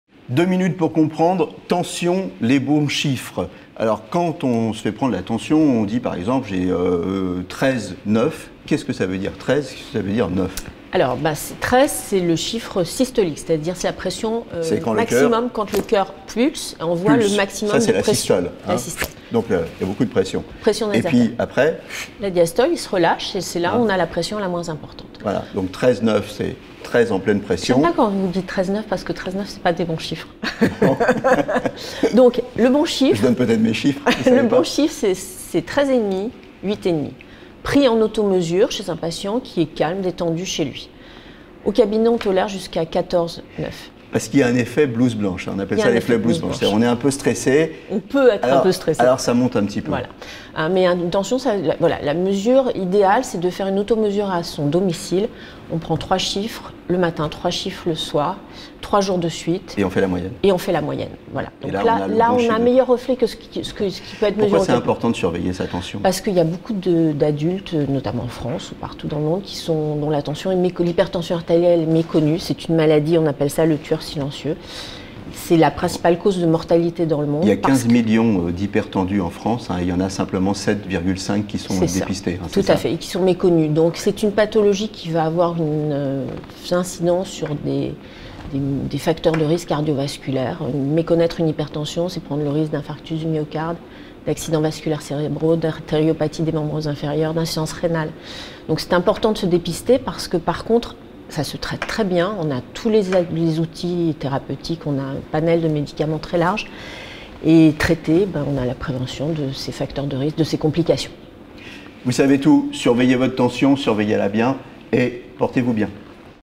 LA TENSION : LES BONS CHIFFRES
2 minutes pour comprendre : LA TENSION : LES BONS CHIFFRES
Dr Michelle Darmon Médecin Généraliste Enghien Les bains